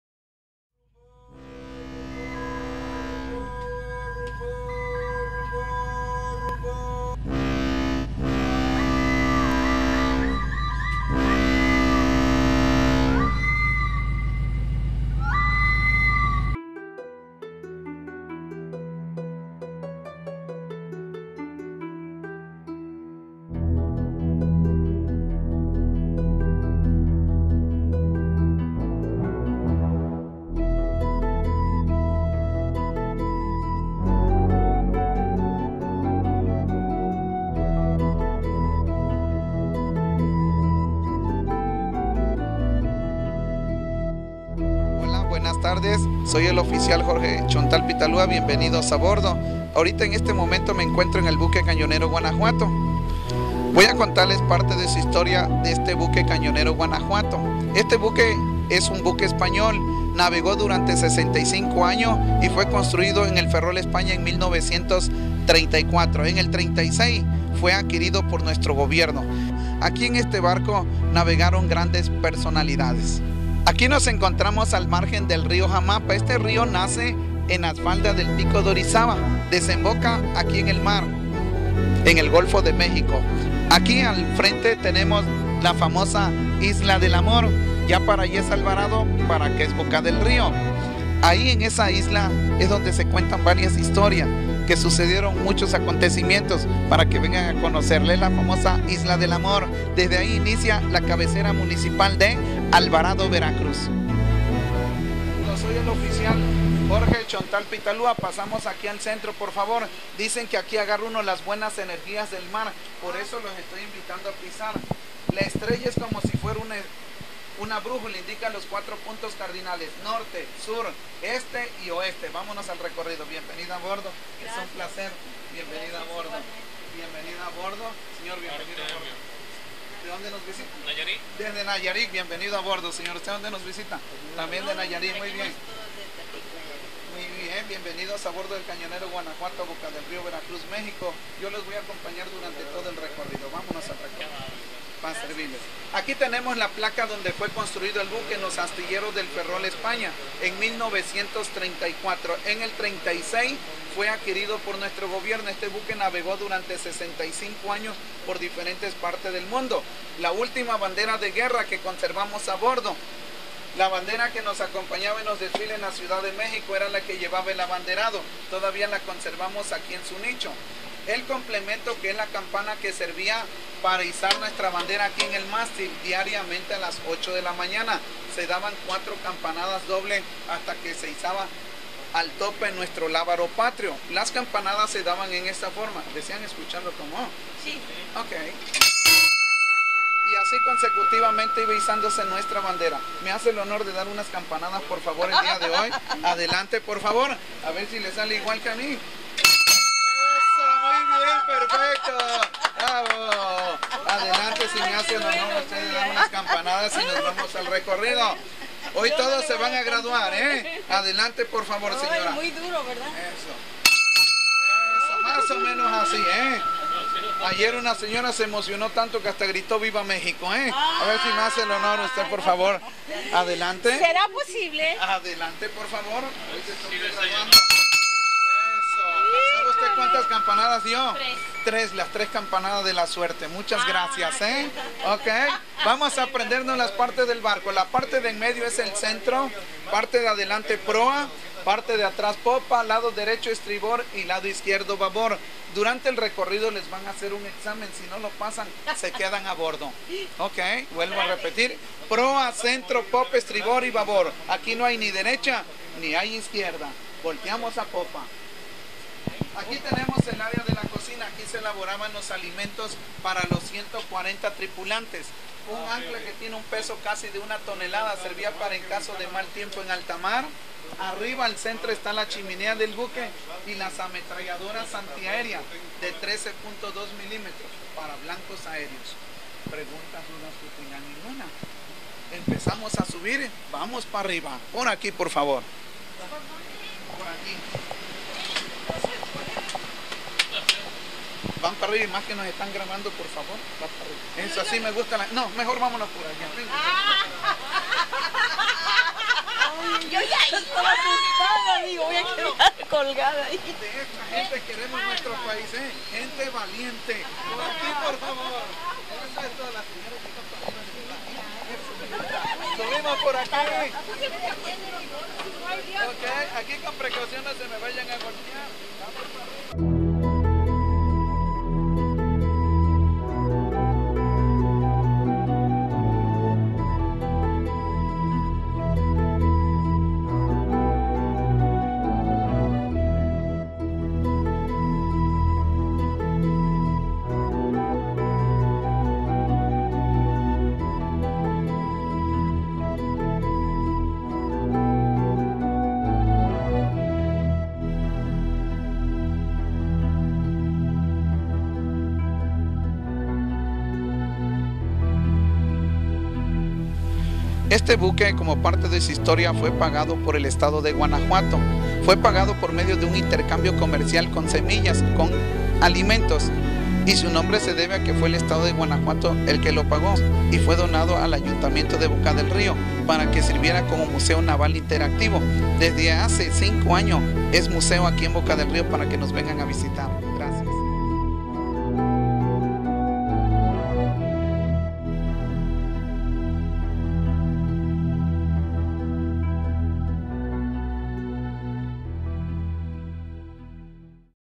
TURISMO BOCA DEL RÍO ( Buque Cañonero C-07)
El Cañonero C-07 Guanajuato es un buque de guerra de ochenta metros de eslora (largo) y doce de manga (ancho) construido en España y dado de alta en la Armada de México en 1934. Trás haber sido retirado del servicio en 2001 es hoy, un Museo Naval Interactivo ubicado en el muelle de la Avenida de Las Banderas, zona centro del Municipio de Boca del Río.Ver.